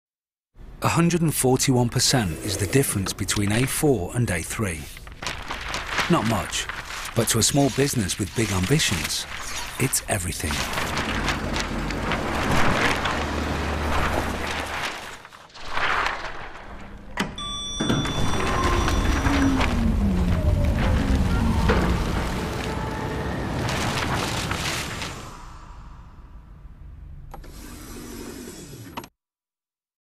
Brother Printers TV Ad "141%" (Architect)
Stop frame TV ad for Brother printers' A3 range.

Creatives: Sam Haynes and Lee Trott.
Director: Noah Harris.